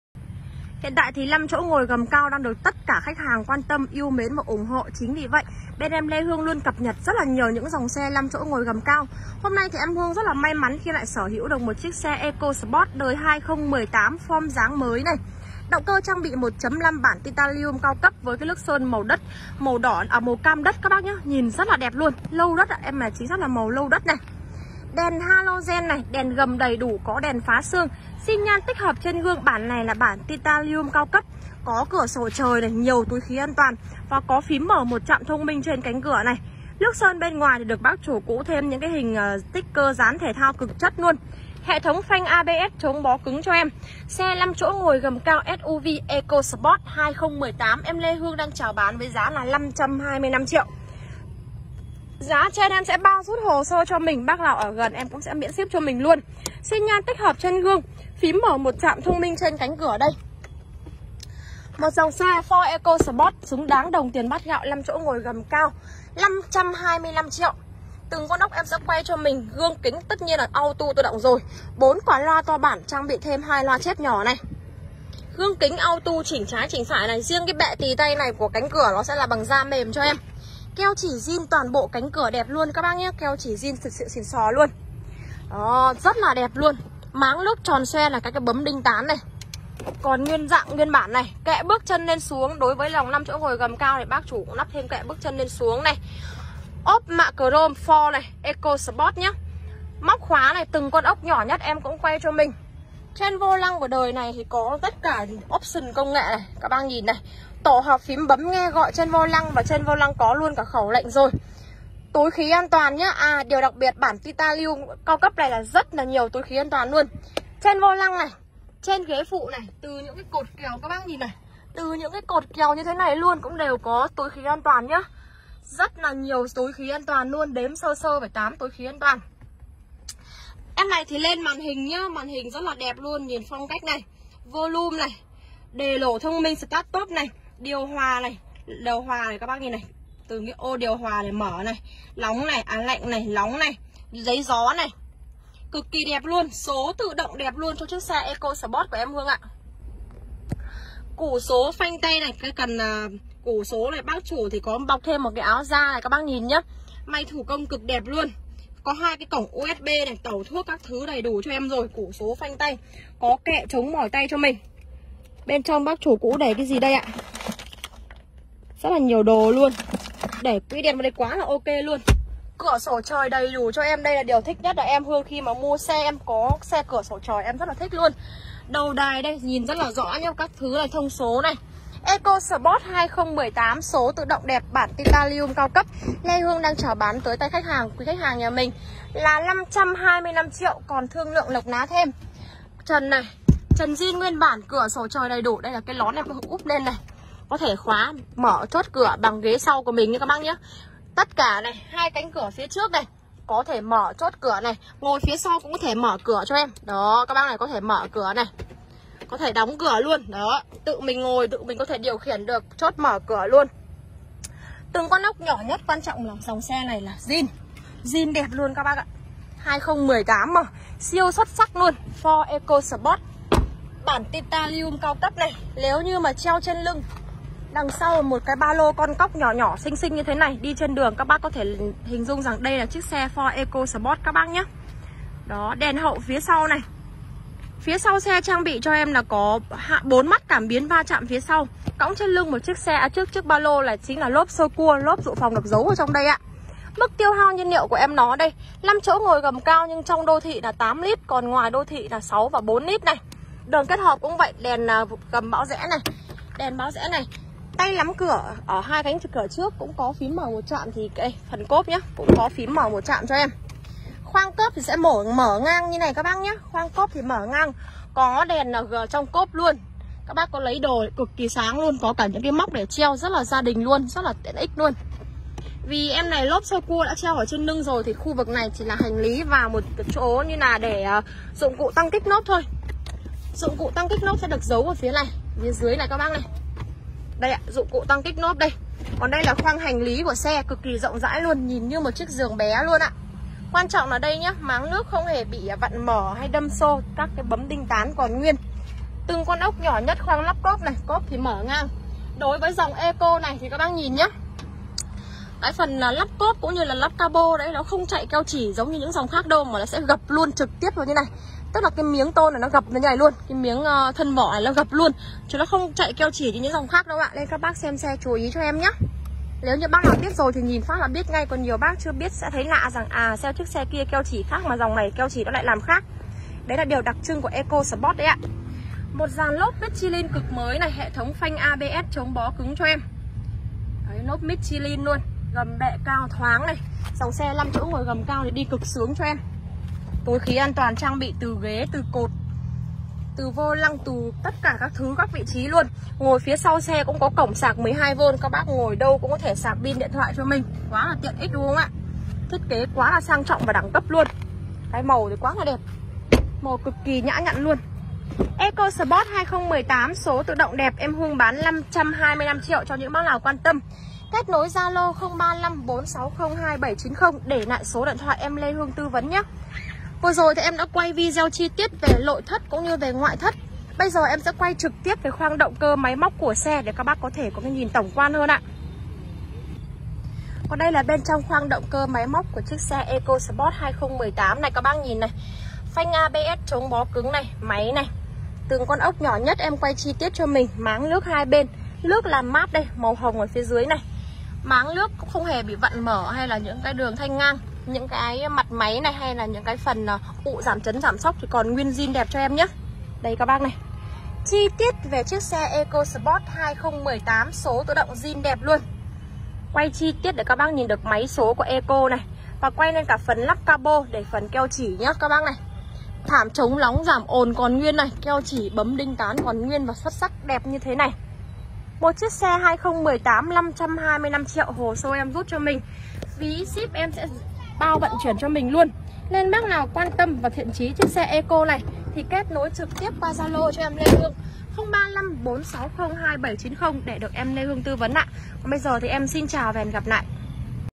Lê Hương vừa về Ecosport gầm cao máy thoáng giá rẻ
Lê Hương vừa về Ecosport gầm cao máy thoáng giá rẻ . LH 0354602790 gặp em Hương chân dài nhé xin cảm ơn.